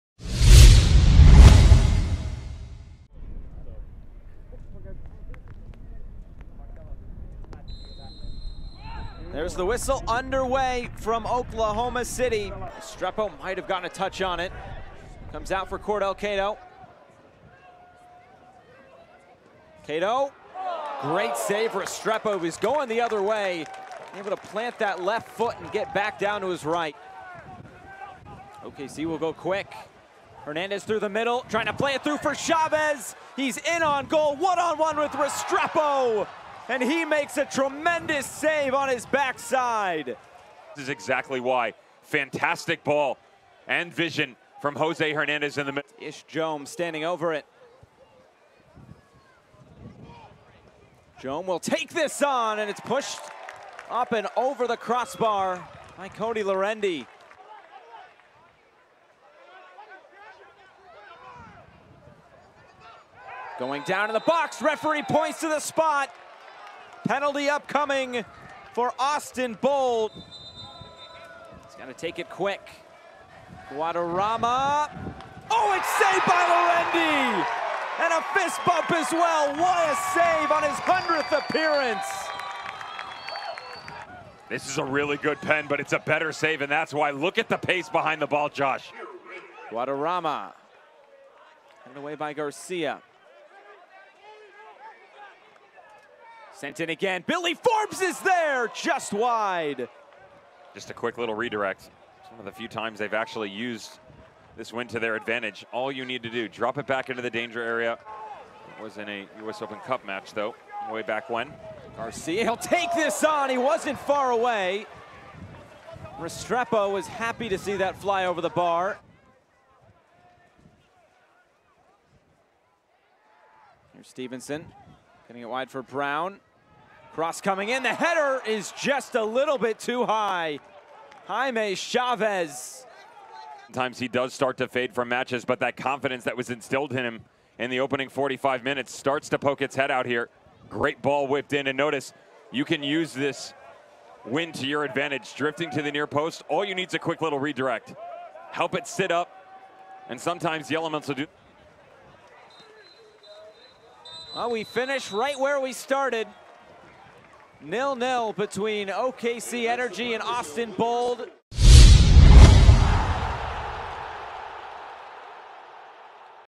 HIGHLIGHTS OKCvATX | 09-26-2020
OKCvATX - Austin Bold FC needed victory to move back into second place in Group D as it visited OKC Energy FC at Taft Stadium in a key game in the race for the 2020 USL Championship Playoffs.